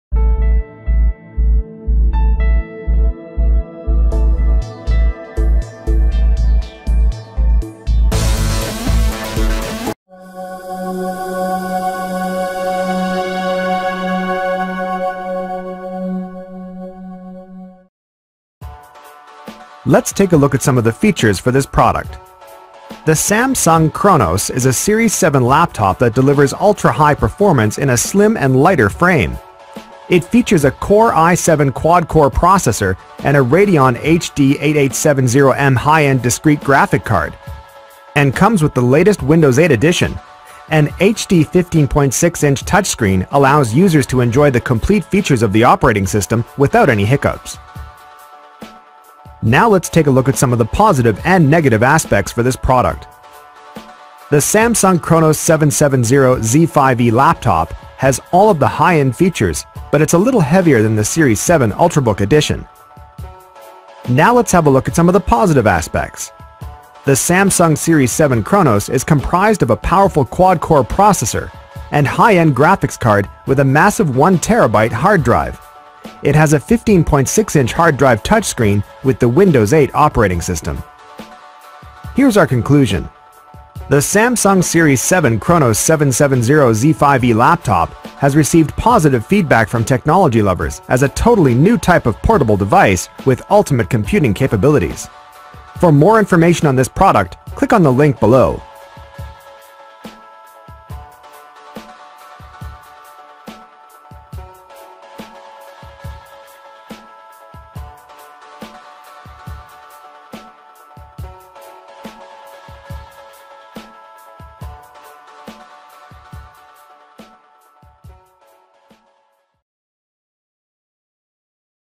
Samsung Chronos 770Z5E Review
Check out our Samsung Chronos 770Z5E Review and find out in what ways the Samsung Chronos 770Z5E will enable you to enjoy blazing fast computing speeds and ultimate graphics.

Samsung Chronos 770Z5E is a series 7 laptop that delivers ultra high performance with exceptionally fast hardware and software solutions and has these amazing features:

* Powered by the most extreme processor and discrete graphics card currently available in the market. It features the Core i7 Quad core processor with a turbo boost of 3.4GHz and 6MB cache. Samsung has integrated a Radeon HD 8870M high end discrete graphics card, the latest on the market and delivers stunning quality 3D graphics for a hardcore gaming experience

* It offers a 15.6 inch full HD touch screen display up to 1920×1080 with 300nit backlit

* It comes with built in 16GB system memory with 1600MHz bus speed, and a massive 1 terabyte hard drive to store HD content

* Features 4 watt JBL stereo speakers with a virtual woofer and an HD 720p cam

* Input/output devices include 2 Super-speed USB 3.0, 2 High-Speed USB 2.0, HDMI, VGA, Mic, 3 in 1 Ethernet port

And Windows 8 Operating System. Just to mention a few!

Do see the video review above for the full story about Samsung Chronos 770Z5E

You can see where to buy the Samsung Chronos Series 7 at the best price and more details including users' reviews about the Samsung Chronos 770Z5E here:

Thank you for finding out about this Samsung Chronos 770Z5E info video